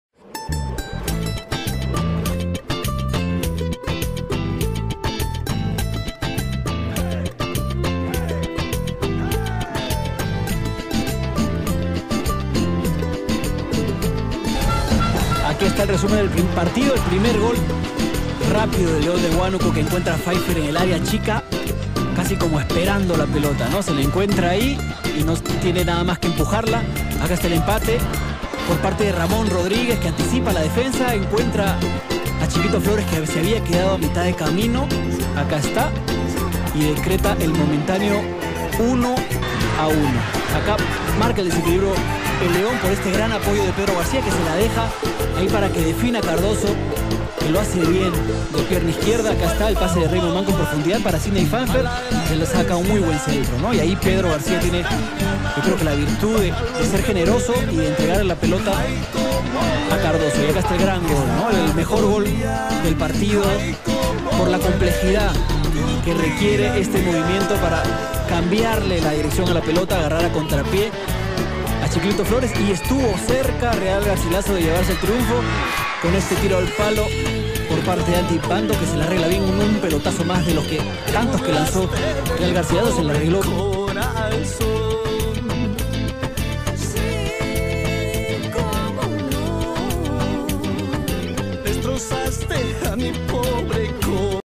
COPA MOVISTAR : LEON DE HCO. VS REAL GARCILASO (2-2)
En un partido regular que ambos tenia ataques de gol , en especial el equipo visitante , leon no pudo y empato con el equipo sensacion del torneo . ENTRA AL FACEBOOK